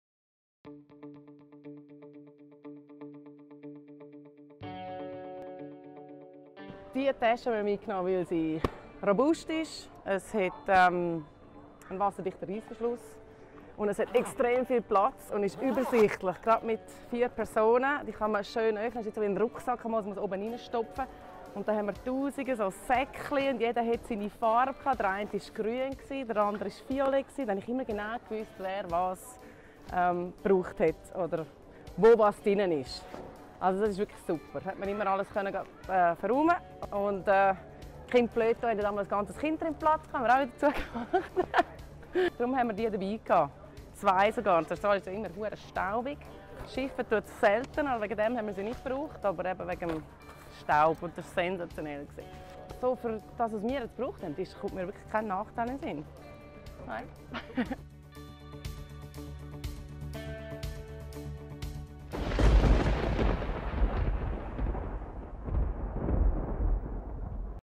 Lieblingsgepäckstück der Familie Blum: Ortlieb Duffel
Sabrina Blum von erklärt, weshalb der Ortlieb Duffel das perfekte Reisegpäck für ein Familienabenteuer in Australien ist.